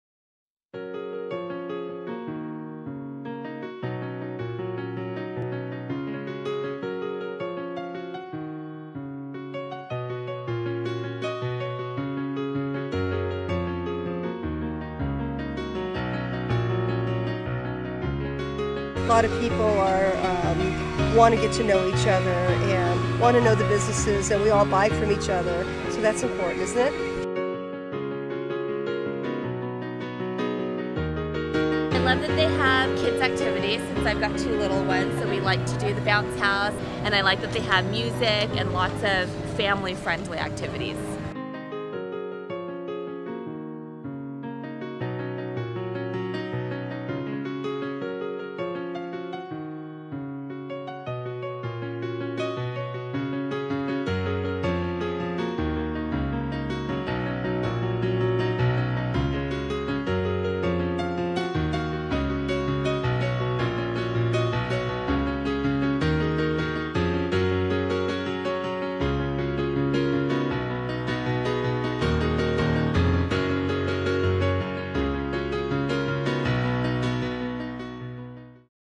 Fullerton Farmers Market
The Fullerton Farmers Market is a fun place to come and hang out with the locals, listen to some live music, eat a ton of pallet pleasing food and relax!